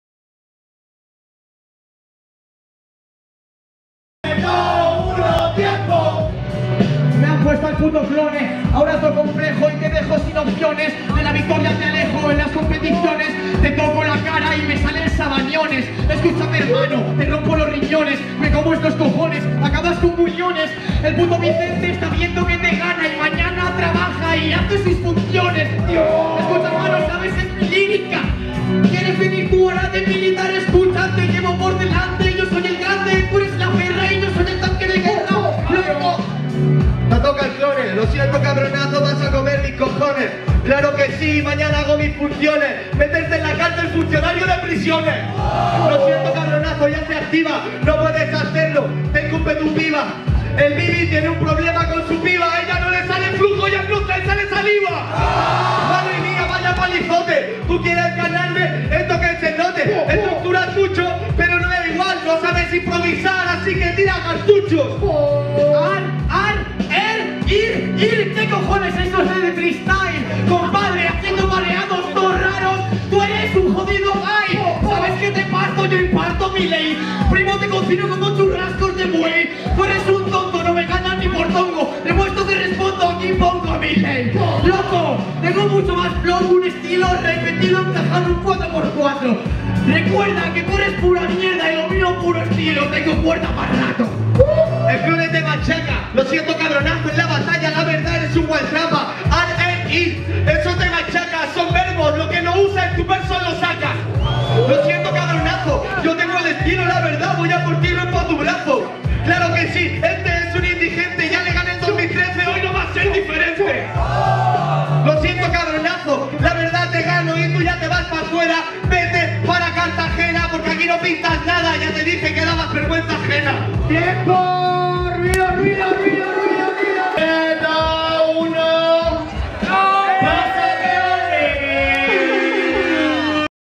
KLONE VS VIVI //OCTAVOS//REGIONAL ALMERIA 2017
🔶8AVOS FINAL REGIONAL DE ALMERIA ENTRE:.
▶KLONE (ALMERIA).
▶ VIVI (CARTAGENA).
🔶GANADOR: KLONE🏆

🔷FACEBOOK: GOLD BATTLE ESPAÑA//GOLD BATTLE SPAIN.